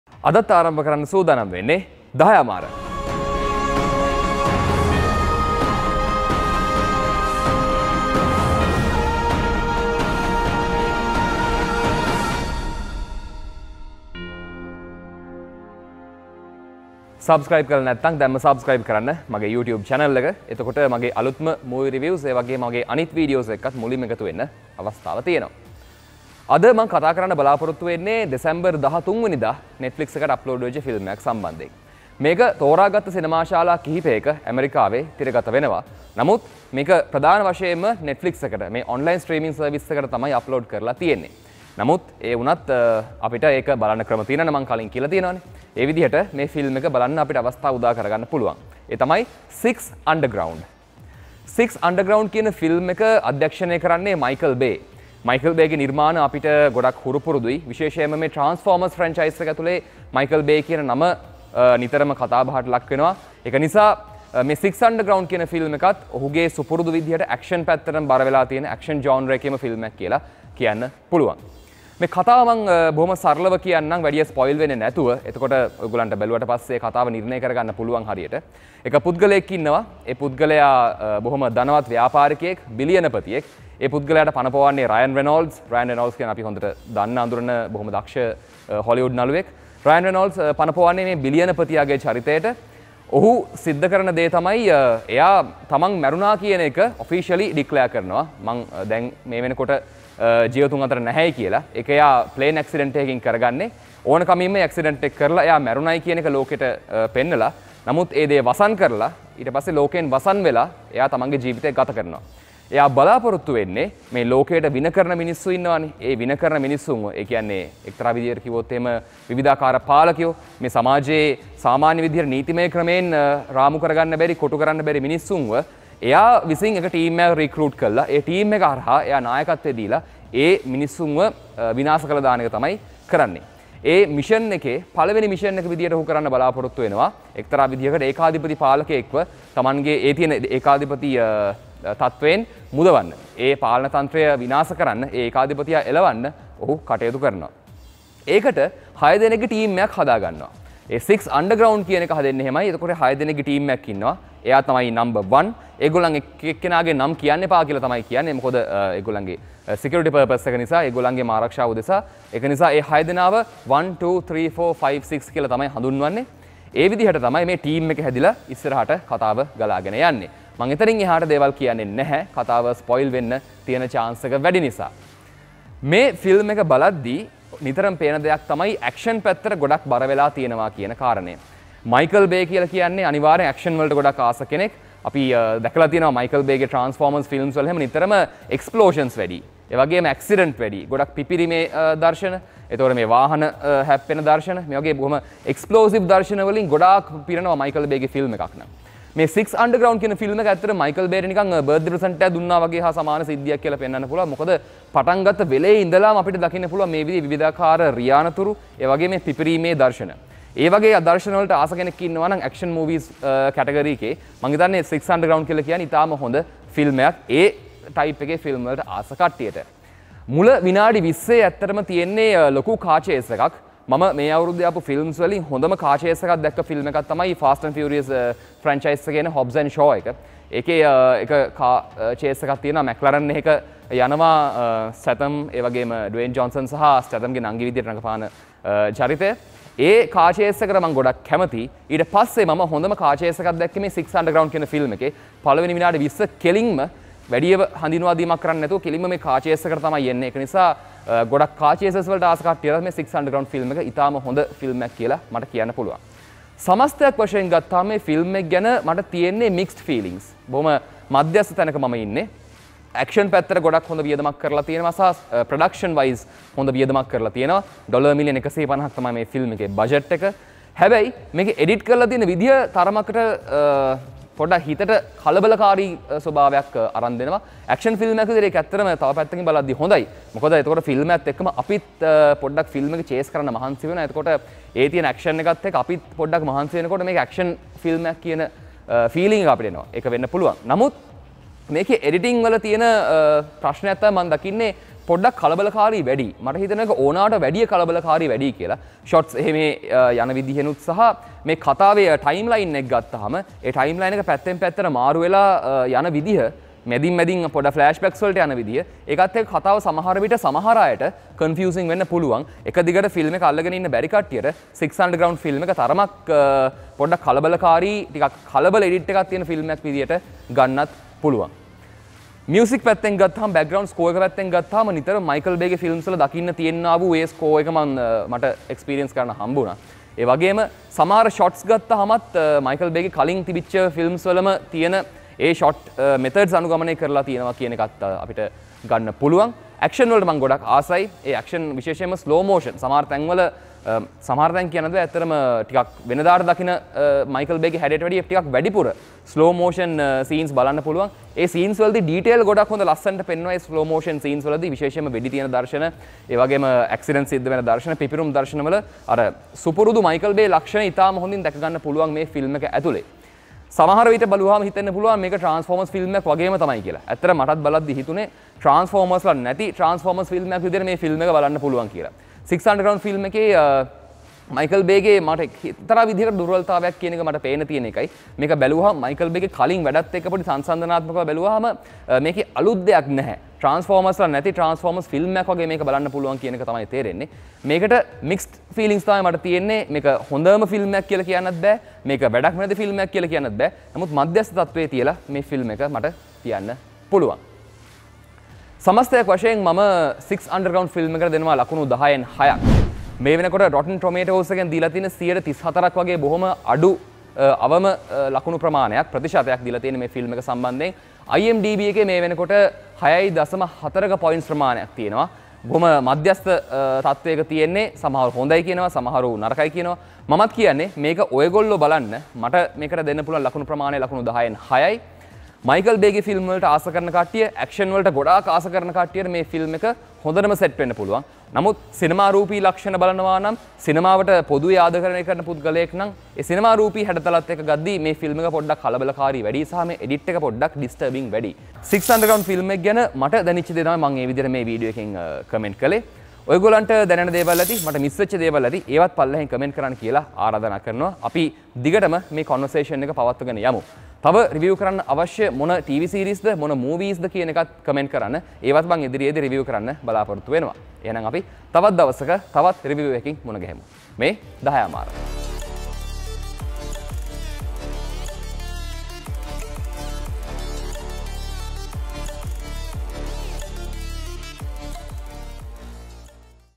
අයියා එළවලා මල්ලි ජනාධිපති වුණා | Dahayaamaara | 6 Underground Review
'6 Underground' is the new film of renowned director Michael Bay, best known for his contributions to the 'Transformers'movie franchise. The movie was distributed via Netflix.
Here, I review about what I felt after watching the movie.